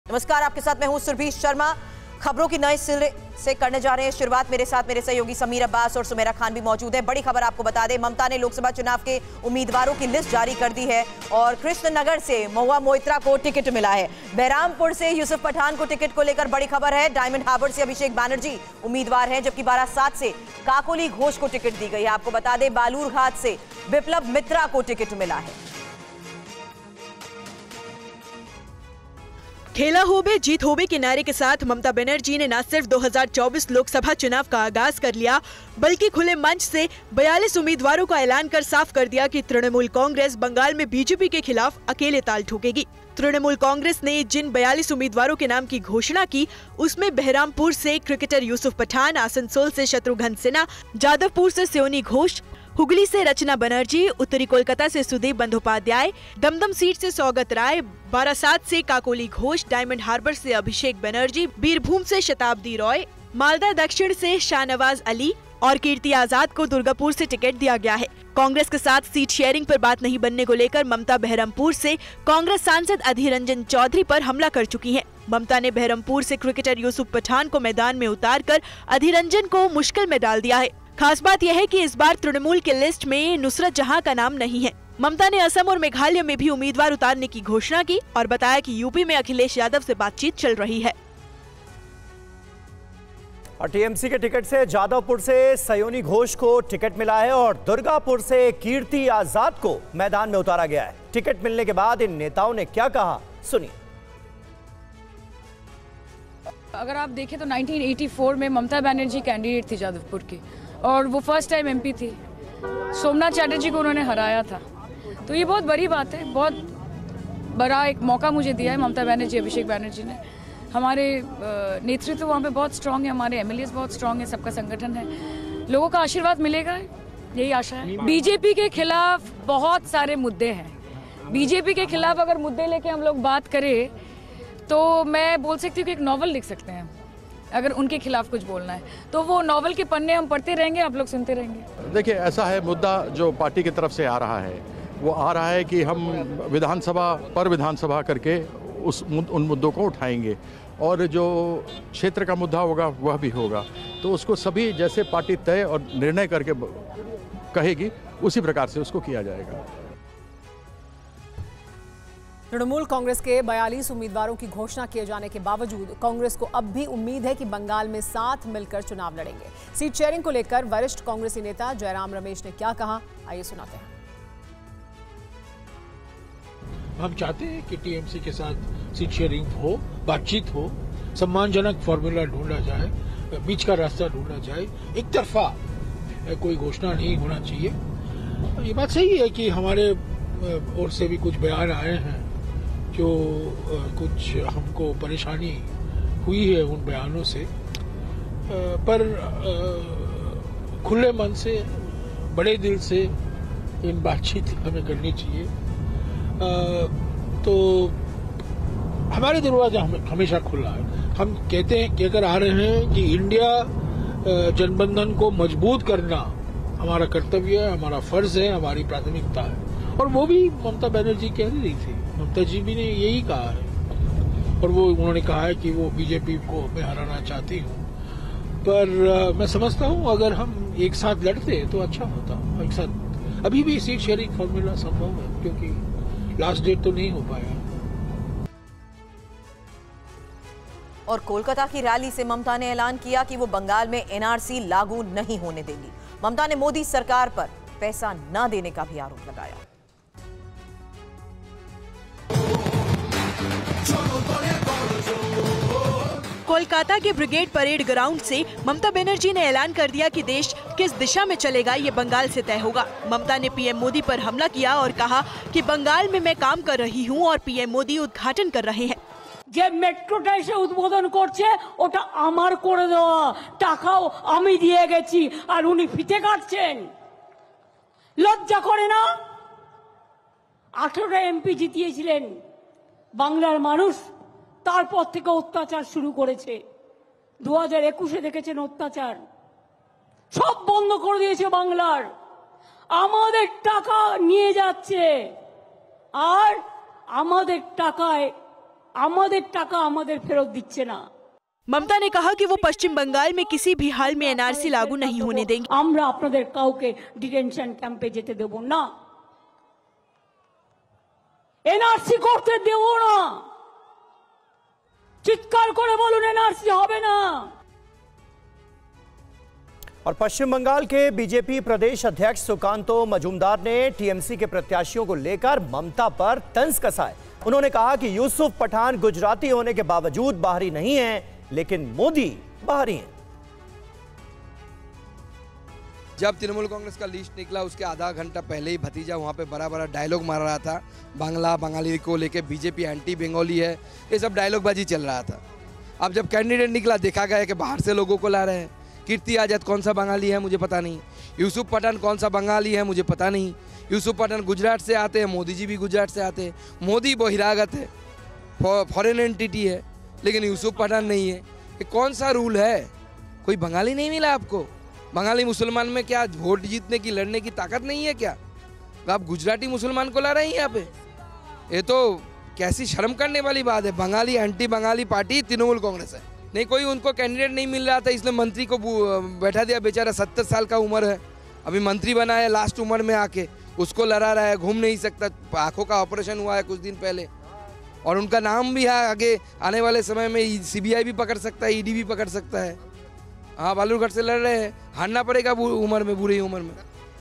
TMC Lok Sabha Election Candidate List: कृष्णनगर से Mahua Moitra को मिला टिकट
ममता ने लोकसभा चुनाव के उम्मीदवारों की लिस्ट जारी की. कृष्णनगर से महुआ मोइत्रा को मिला टिकट. बहरामपुर से यूसुफ पठान को टिकट. डायमंड हार्बर से अभिषेक बनर्जी उम्मीदवार. बारासात से काकोली घोष को टिकट. बालुरघाट से विप्लव मित्रा को मिला टिकट.